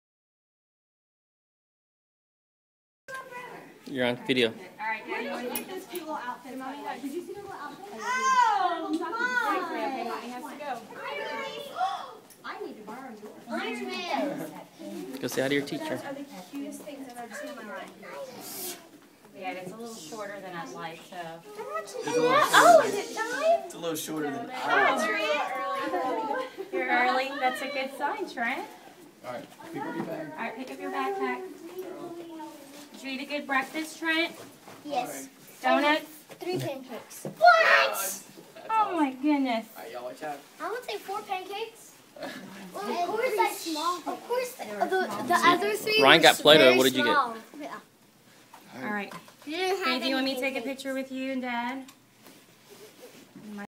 Trent Elofson second grade
Trent Elofson first day second grade. 9/2012